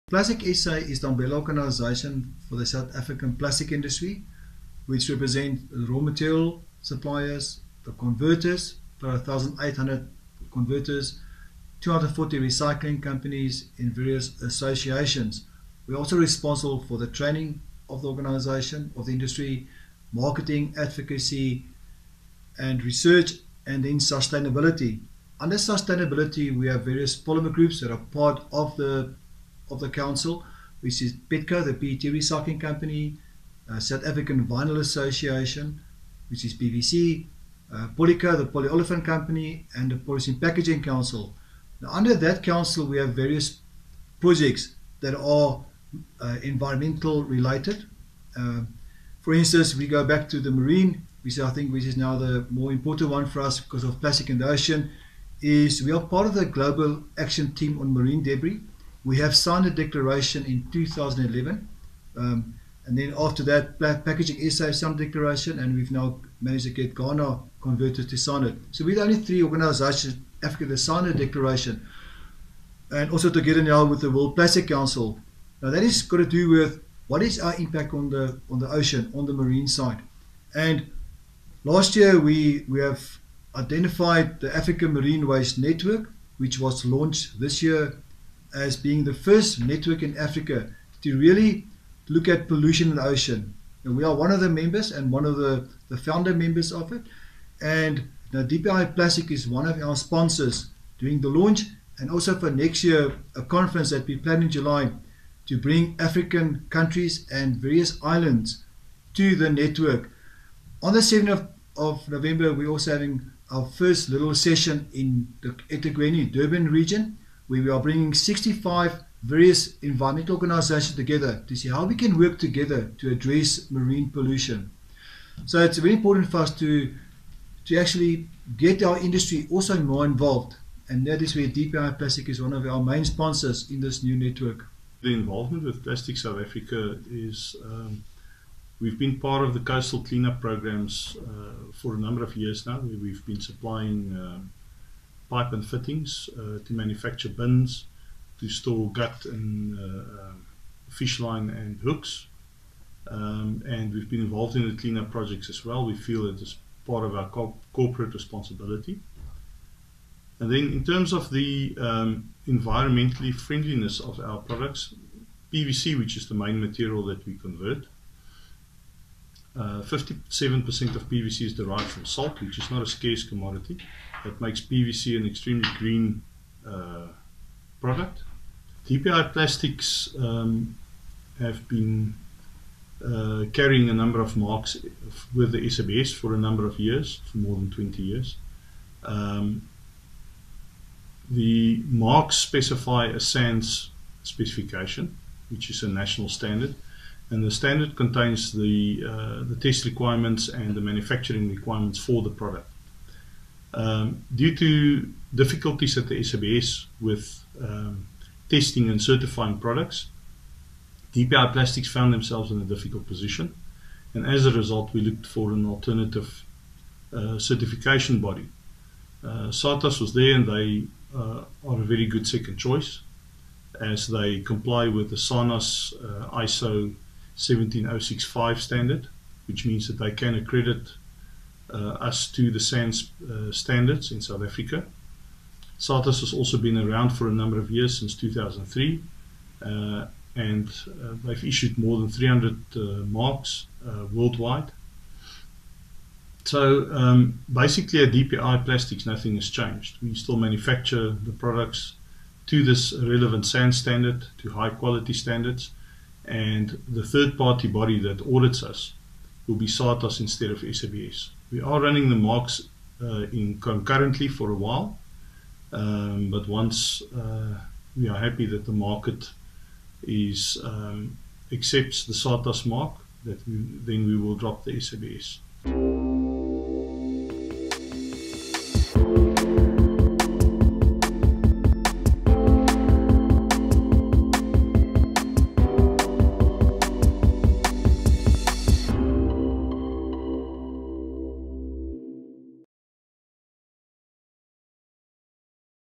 Company supports plastic waste management
As part of its corporate social responsibility (CSR), plastic pipes manufacturer DPI Plastics will sponsor the inaugural African Marine Waste Network (AMWN) conference next year.